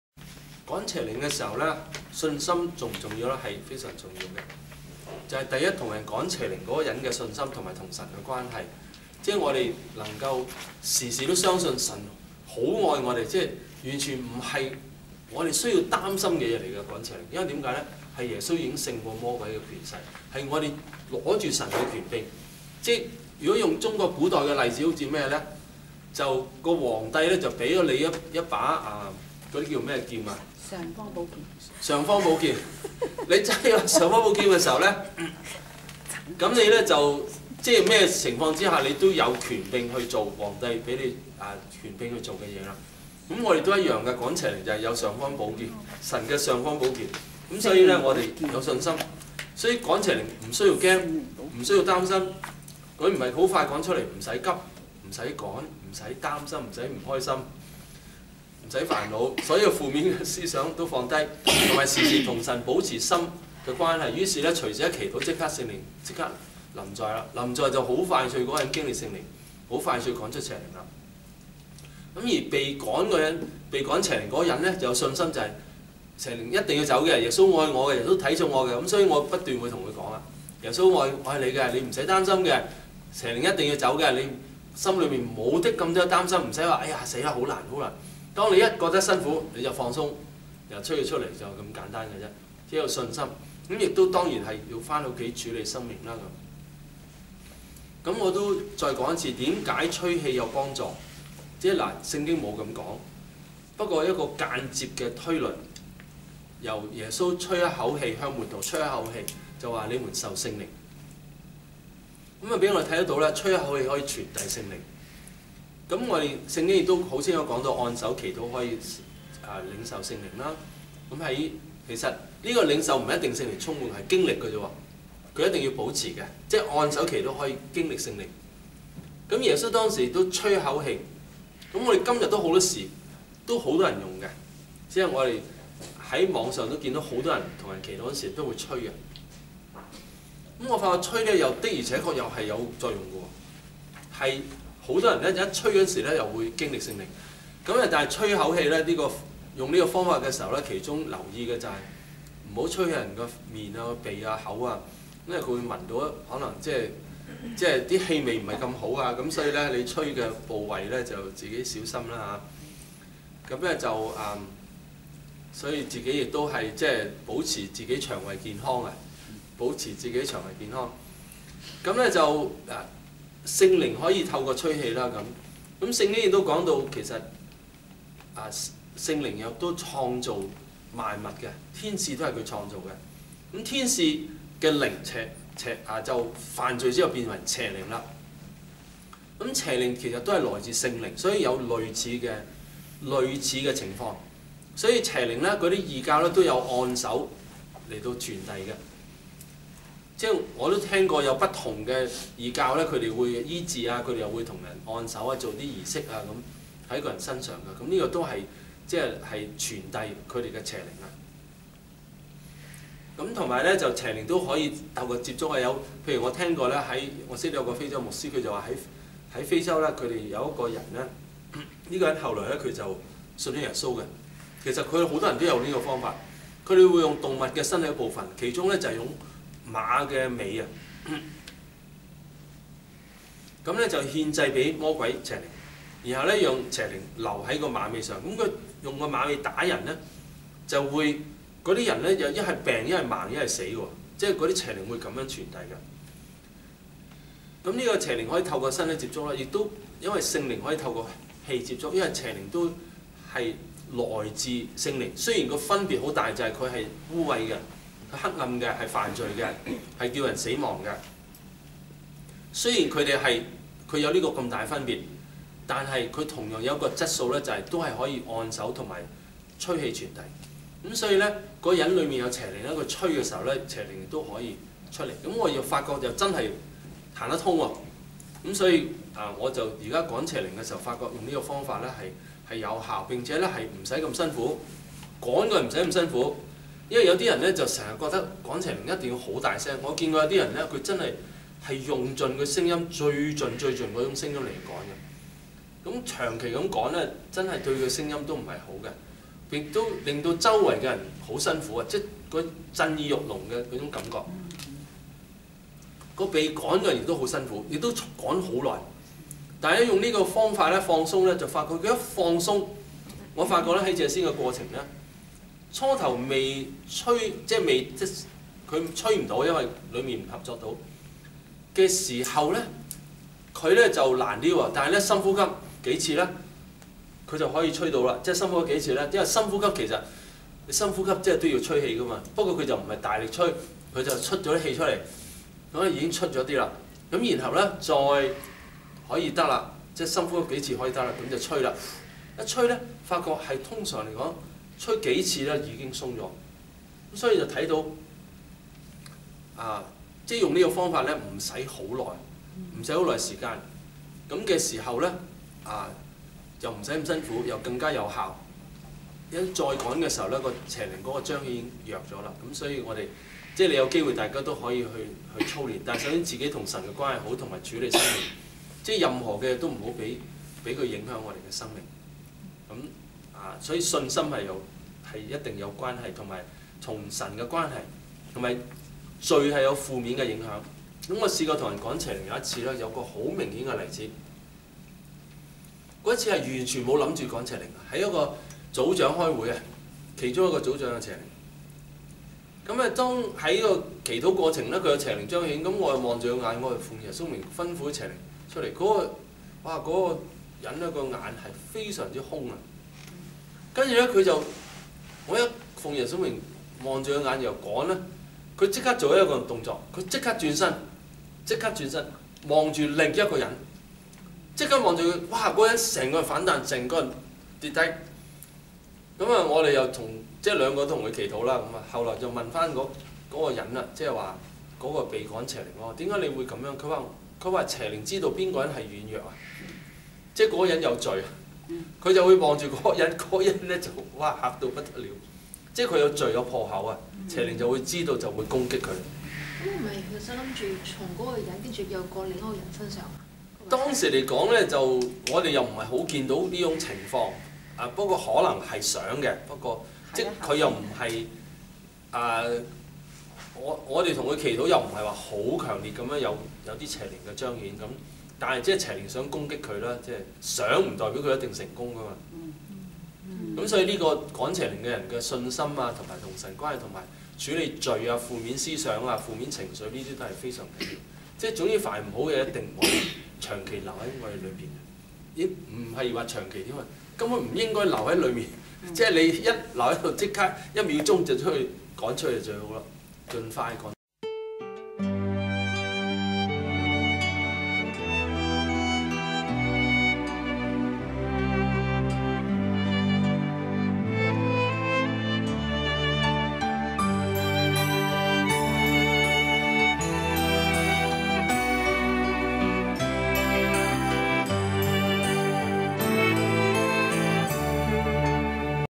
門徒訓練-信心和與神的關係有助釋放邪靈 葉牧師
門徒訓練-信心和與神的關係有助釋放邪靈
1．星期日：3:15崇拜；2:30服事禱告；崇拜後有小組及侍奉訓練課程。
2．星期一：4時服事禱告，7時侍奉訓練課程。